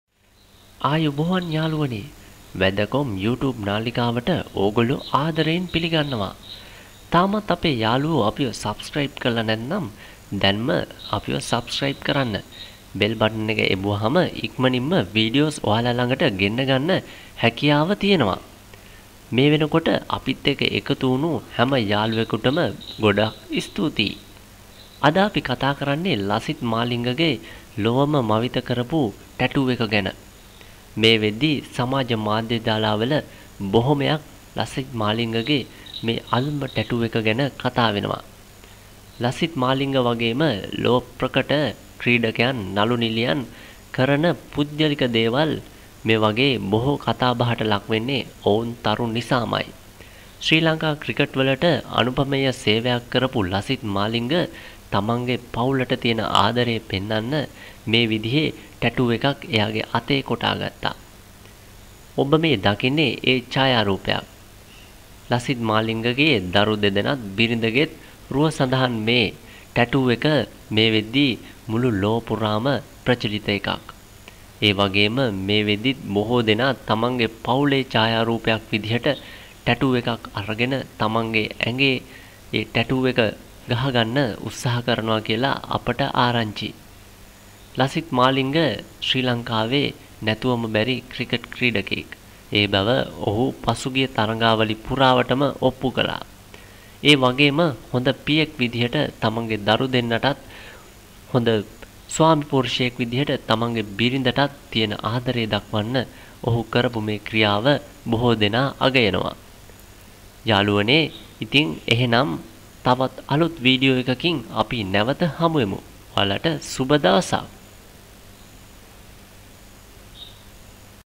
lasith malinga new tatoo - මාලිගේ අලුත්ම ටැටූ එක ලොව මව්ත කරයි
one of the leading cricket players in the world lasith maling have make another fantastic tatoo just to show his love to family. this picture does shows every thing and it was amazing how malinga do expose him self to family.